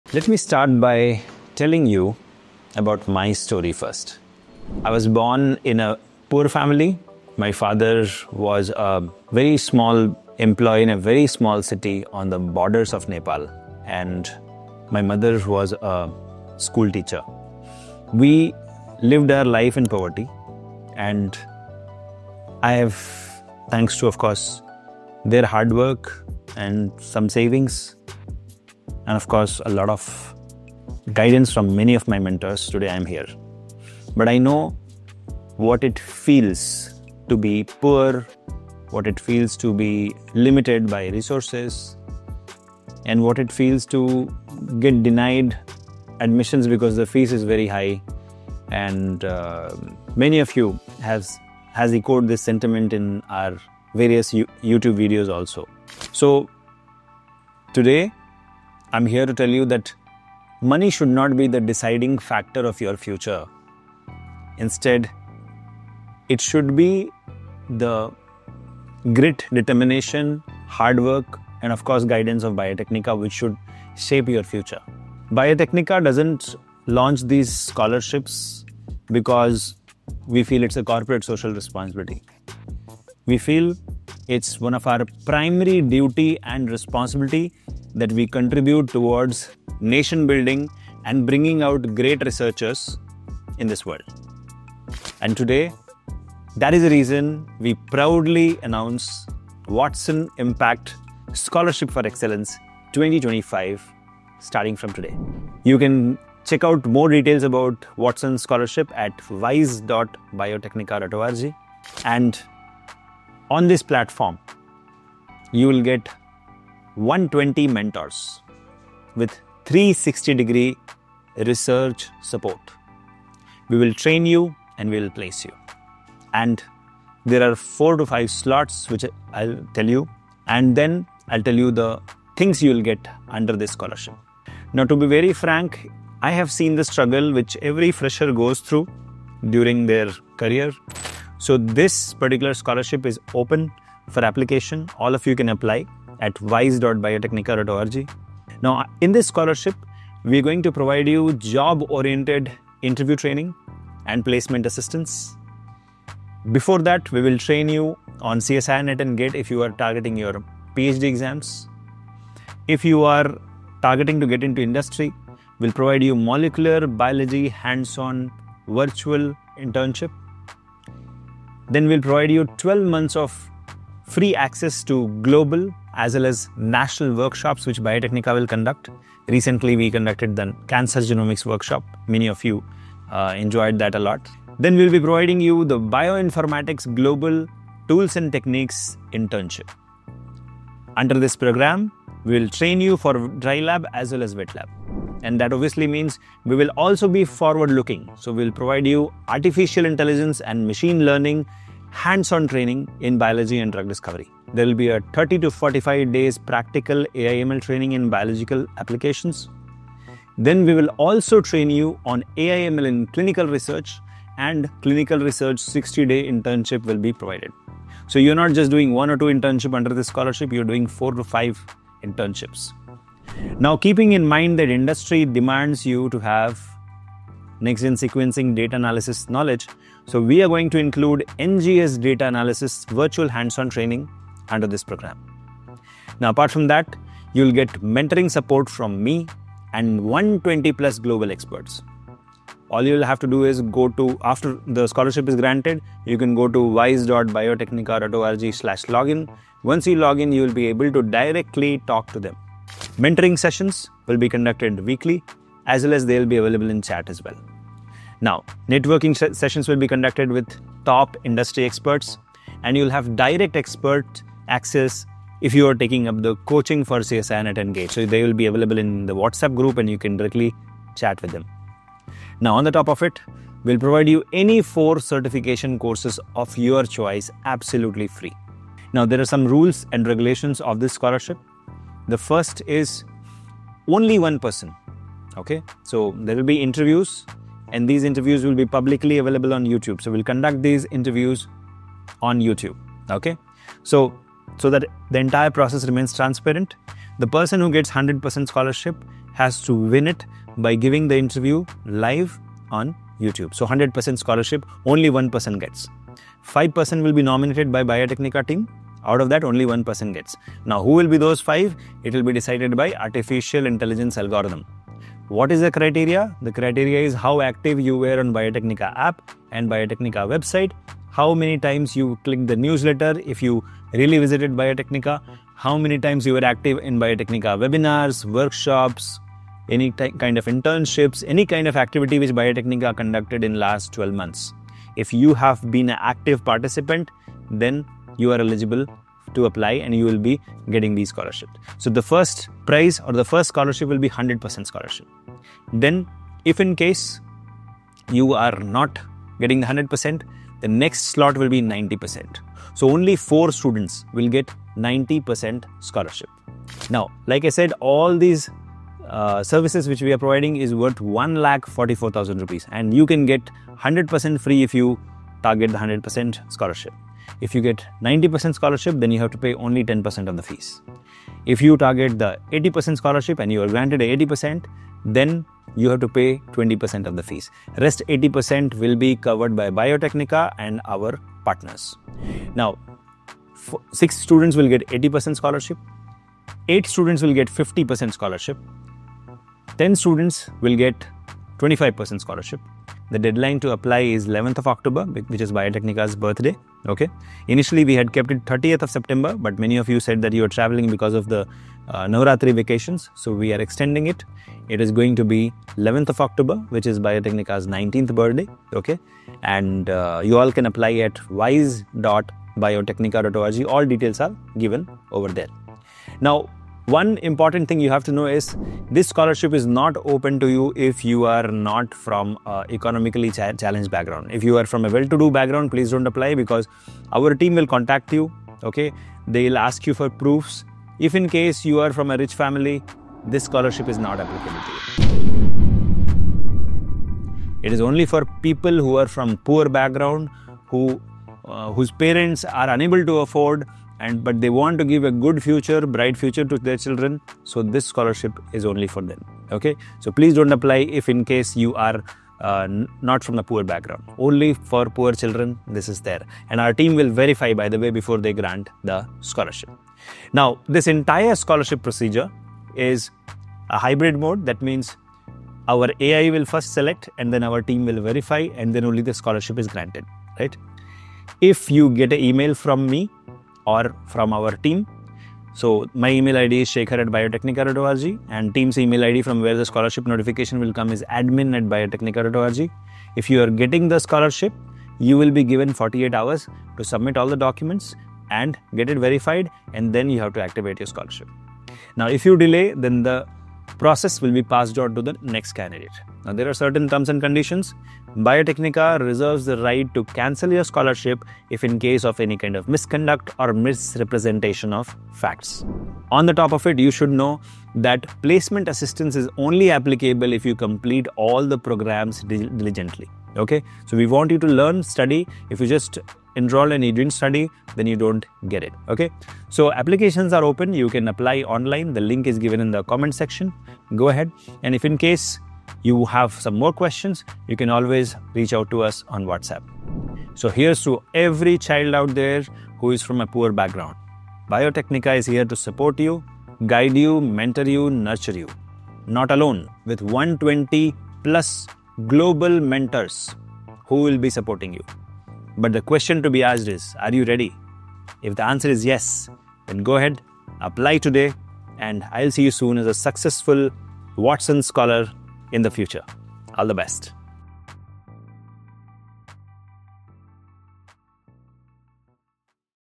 Watson Impact Scholarship 2026 | Scholarships, Internships & Mentorship | Biotecnika
Watson Impact Scholarship 2026 – Could This Be Your Chance? 🌟
Imagine getting world-class biotech training, hands-on internships, mentorship from 120+ global experts, and placement support… all without worrying about fees! Sounds unreal? Well, it’s real – and it could be YOUR future!

The Watson Impact Scholarship 2026 is specially designed for talented students from economically challenged backgrounds who are ready to work hard and make a mark in biotechnology.

🚨 Deadline: 11th October 2026
👉 Don’t miss this chance!

Why You Should Apply:
100% scholarship for one lucky student
Job-oriented interview training & placement assistance
Virtual molecular biology & bioinformatics internships
AI & ML in biology training
Clinical research & NGS data analysis practical experience
Access to 12 months of workshops & global networking